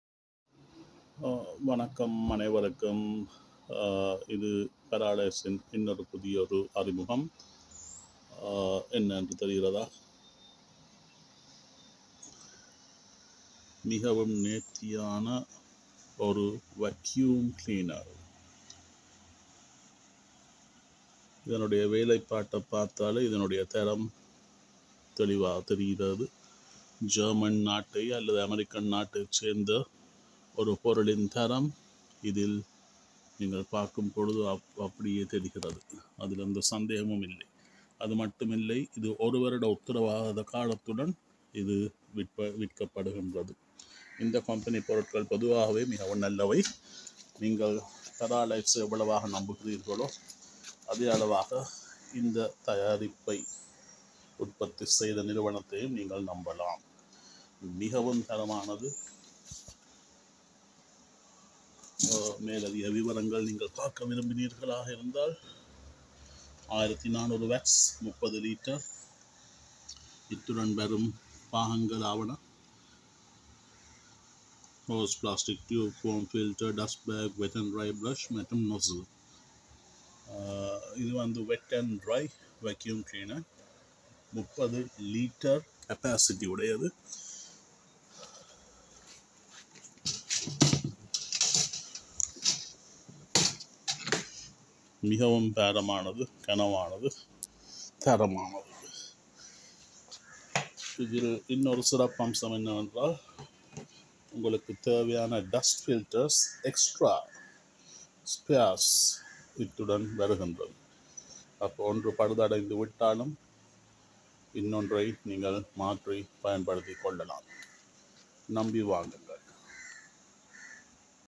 30 Litre Wet & Dry Vacuum
30 Litre Wet and Dry Astro Vacuum Cleaner and Blower with SPARE FILTERS and ONE YEAR WARRANTY - 22900/=

Voltage – 240V
Frequency – 50Hz
Capacity : 30L
Power : 1400W
Vacuum : 17Kpa
Meas: 35.5*35.5*51 Cm

Para Lights Jaffna
115 Chemmani Road
Near Kittu Poonga